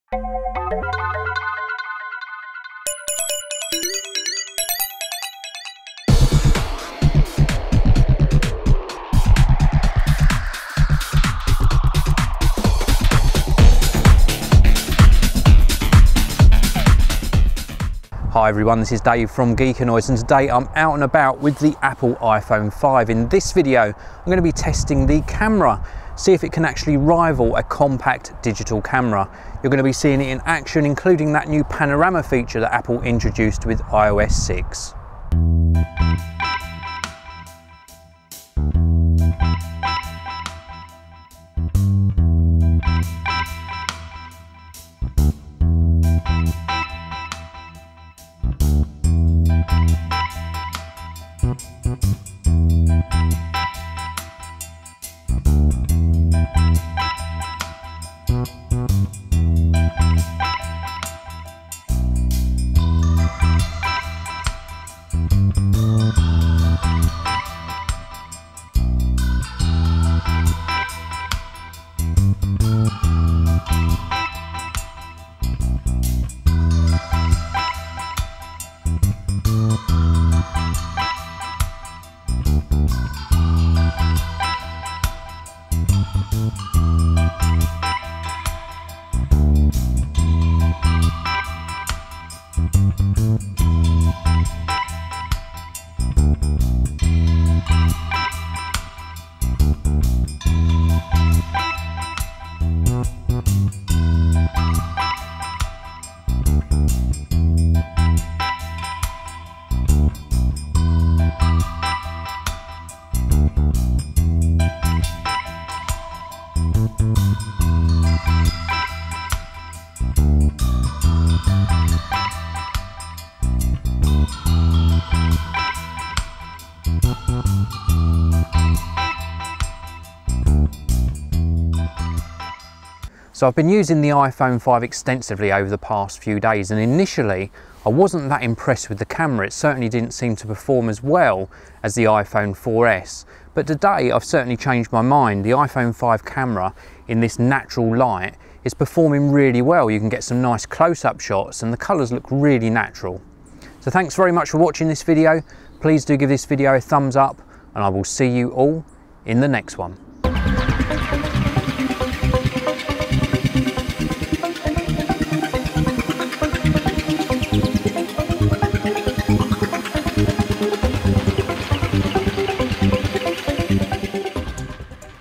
iPhone 5 Photography Test | Geekanoids
Apple iPhone 5 Photography Test | Geekanoids ... after a restore on the iPhone 5 the camera now seems to be performing as expected. Join me on this photography trip to thoroughly test if the iPhone 5 can match up or even replace a compact digital camera. This test includes a look at the Panorama feature.